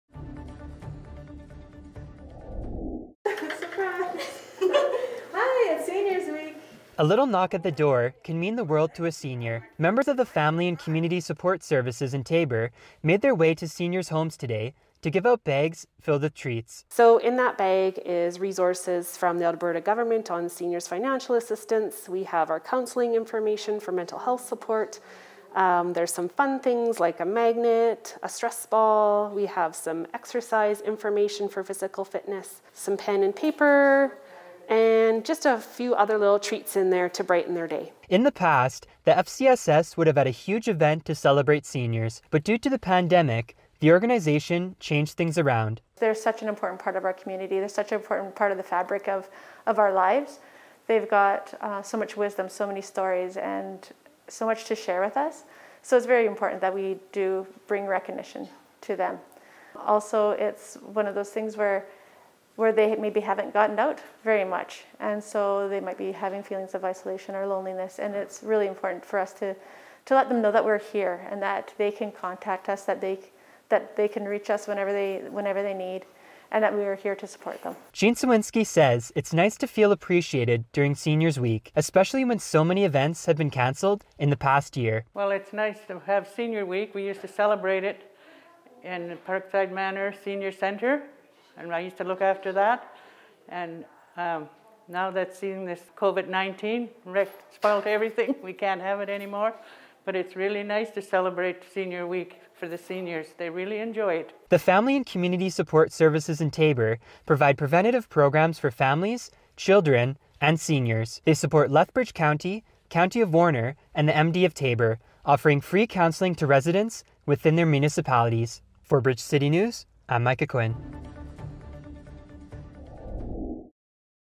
Seniors' Week Begins With Goodie Bags For Seniors - June 8, 2021 - Micah Quinn
Seniors play a huge role in our lives, and June 7 to 13 marks Seniors’ Week to honour those who paved the way before us. As Micah Quinn reports, a Taber organization was out at senior homes today to hand out goodie bags to our most vulnerable.

Aired on: Tuesday, June 8, 2021